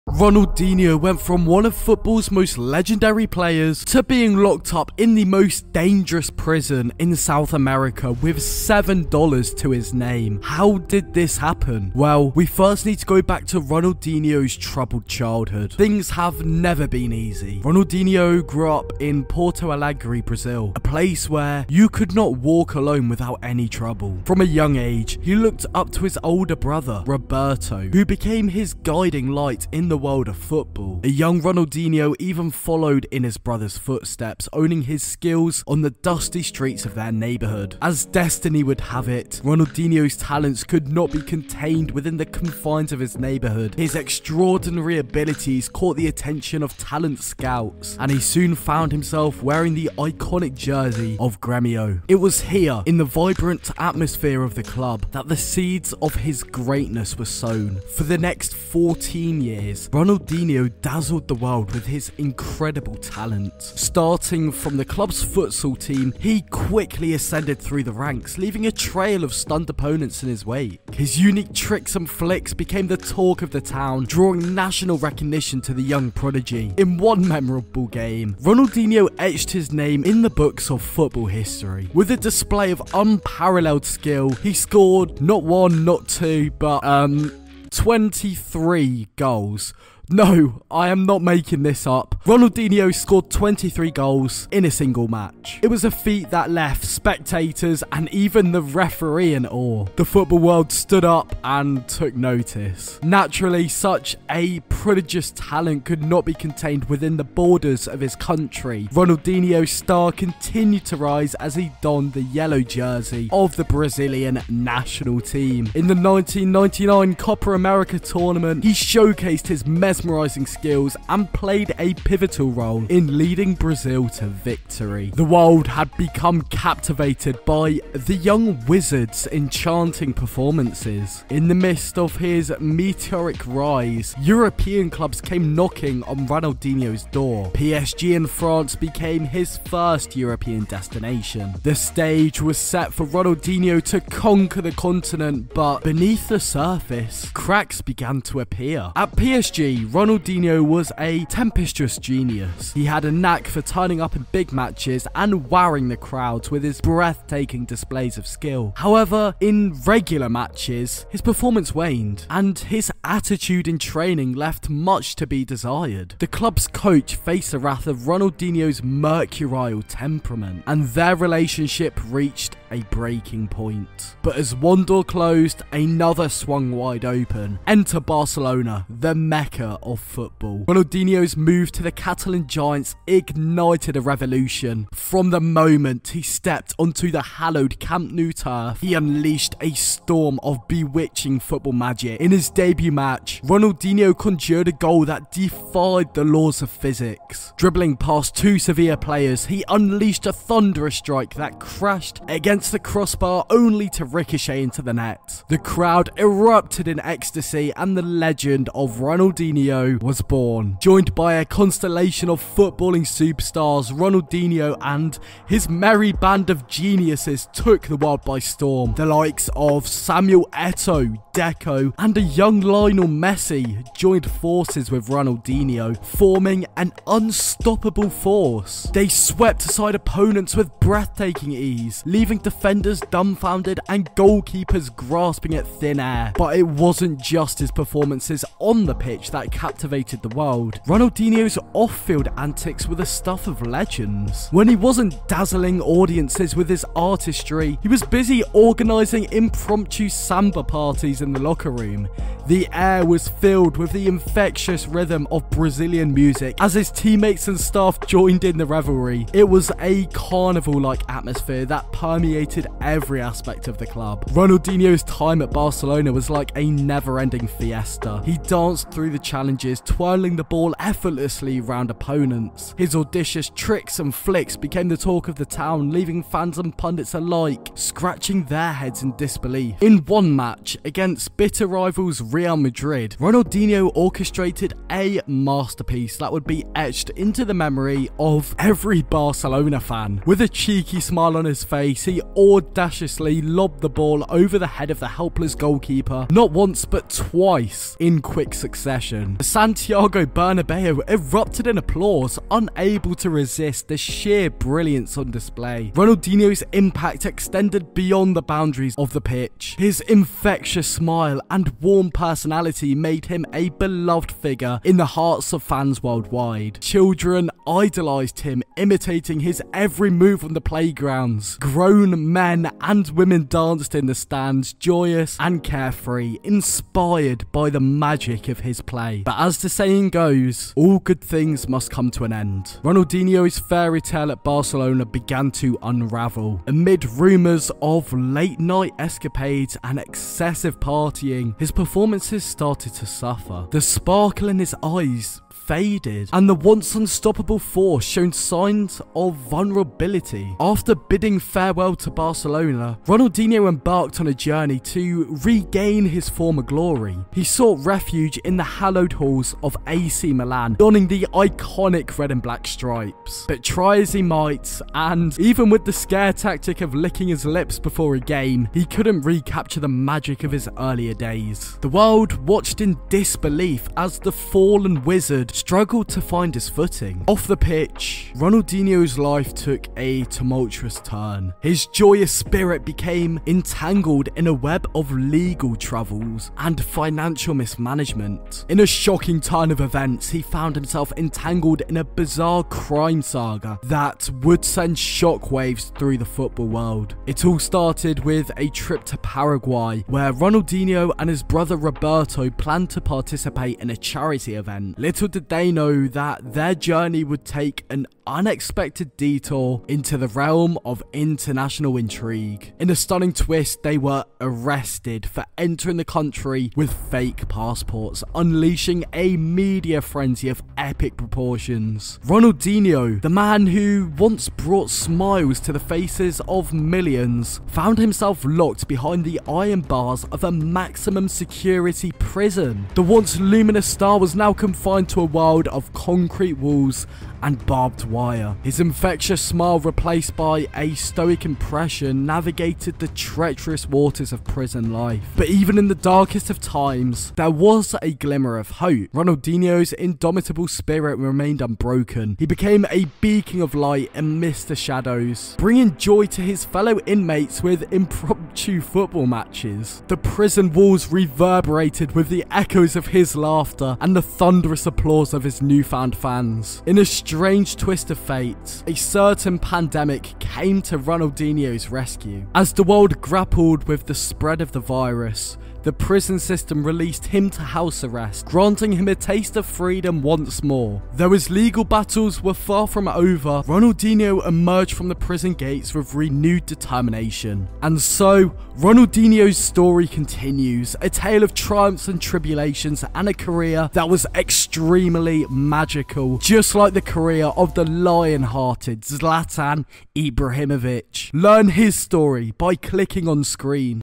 The Rise And Fall Of Ronaldinho: From Icon To Prison
Ronaldinho went from one of football's most LEGENDARY players to being locked up in the most dangerous prison in South America with SEVEN dollars to his name! How did this happen?

0:00 Intro, Leave A Like!
0:13 Troubled Childhood
2:17 Brazilian Wizard at PSG
2:57 Magic, Controversy and Tragedy at Barcelona
5:57 The 'Beginning of the End' at AC Milan
6:27 Legal Battles
7:02 Behind Bars in a Maximum-Security Prison
8:00 What's next for Ronaldinho?